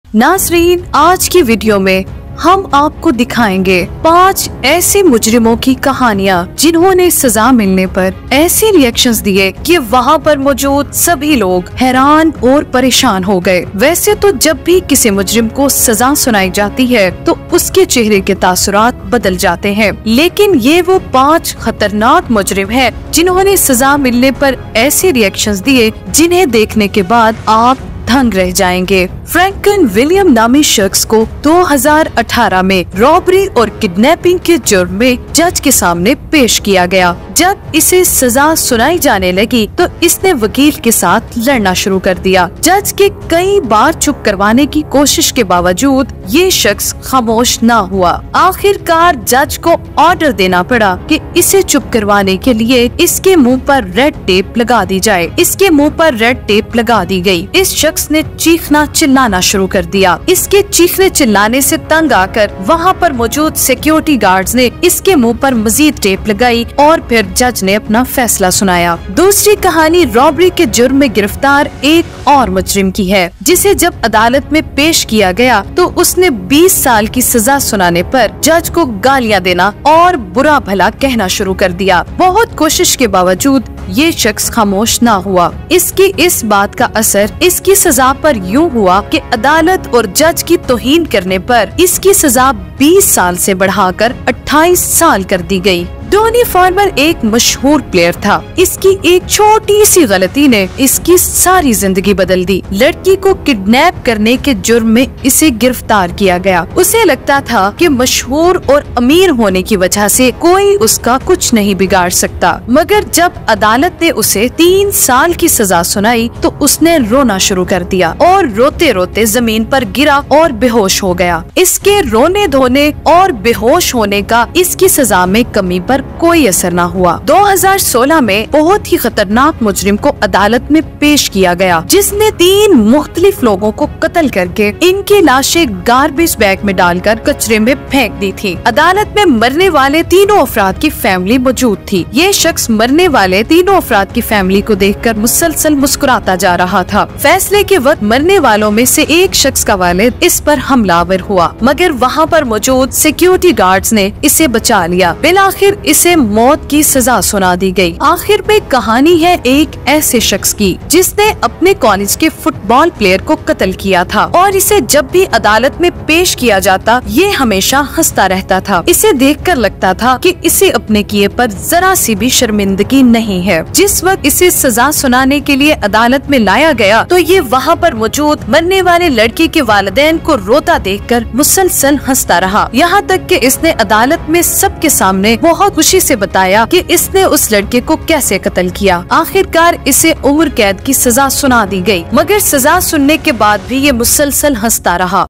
World's Top Five Gangster's Reactions In The Court Room After The Decision By Judge
In this video top five gansters from all over the world gave weird reaction after judge's decision and people sitting in the court Room was shocked